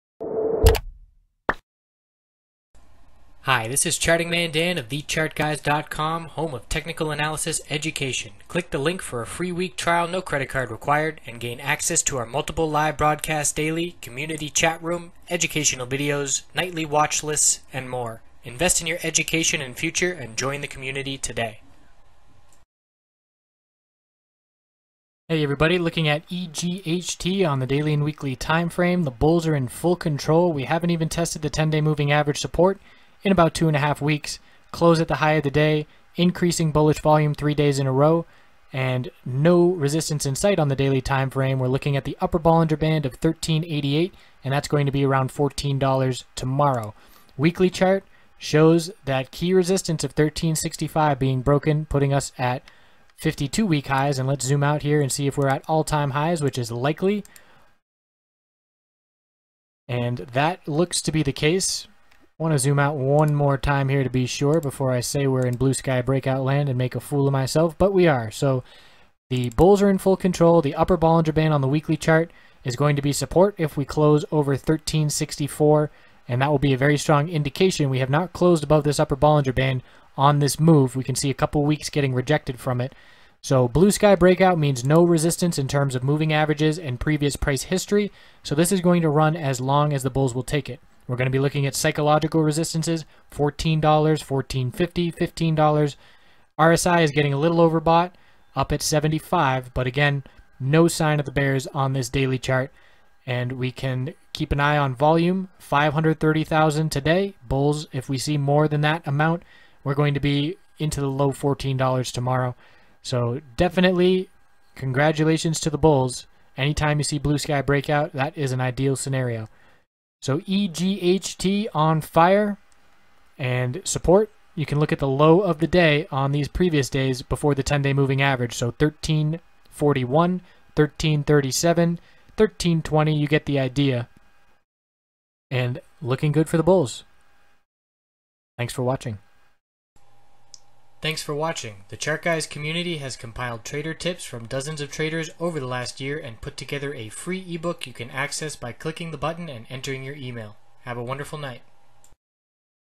EGHT Technical Analysis Video 6/7/2016
A look at the daily and weekly EGHT charts.
Questions, comments welcome.